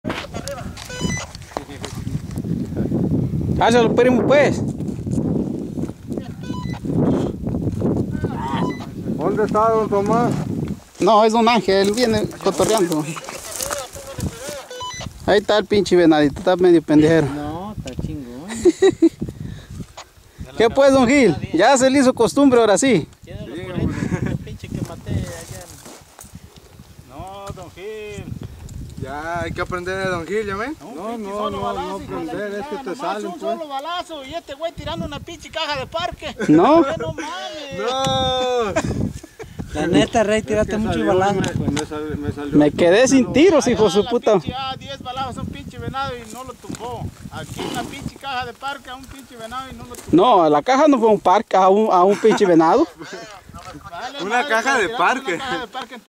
caseria de venados ,don Gil y su venado.mpg
el segundo venado de don Gilverto despues de 4 temporadas que no le salia ninguno ,.